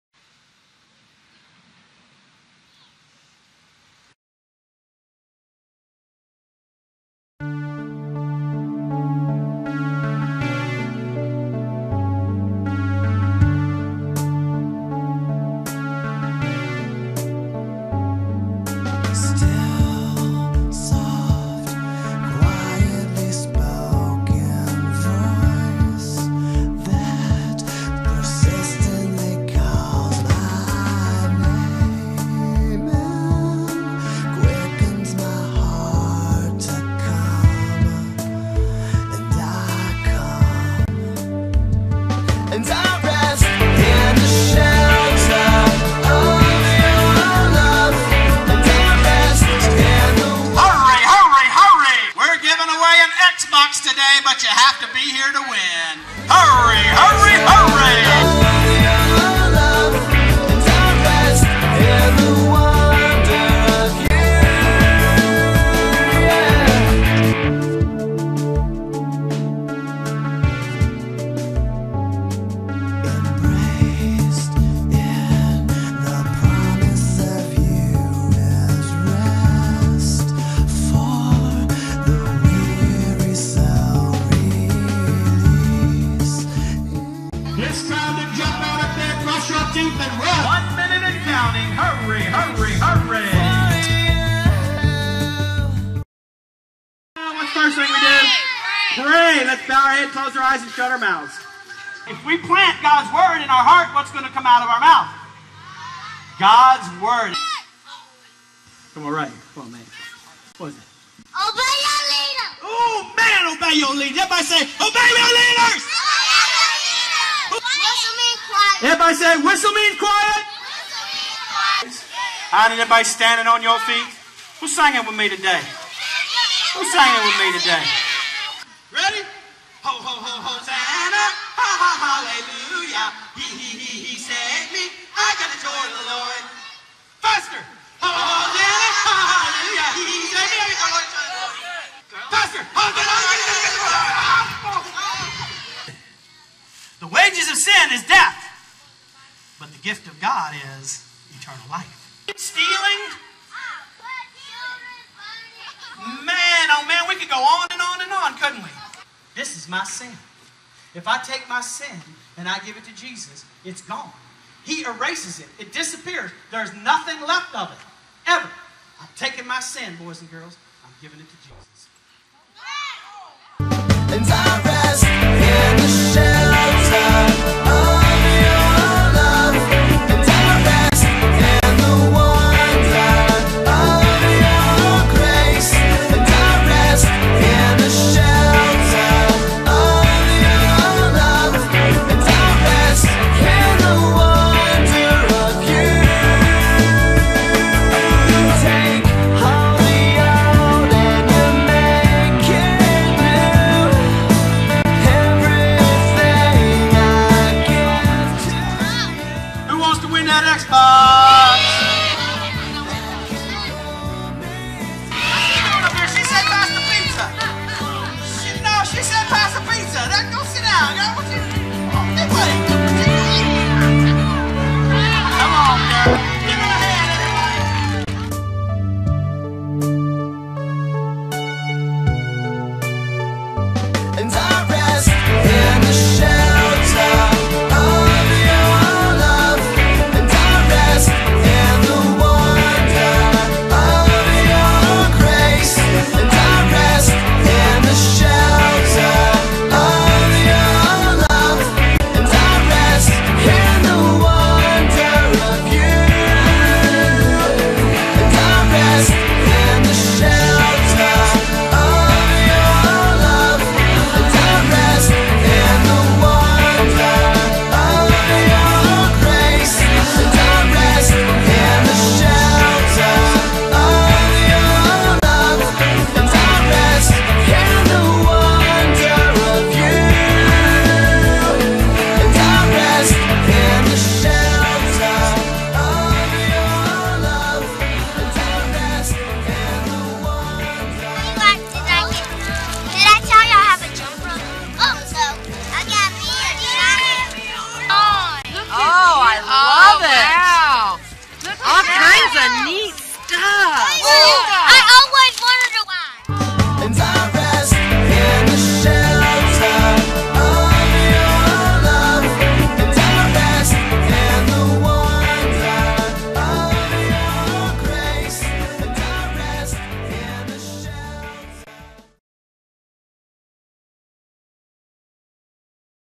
Christmas Shoe Box Give Away
Shoebox Christmas presents at Sidewalk Sunday School. See the blessing on the children's faces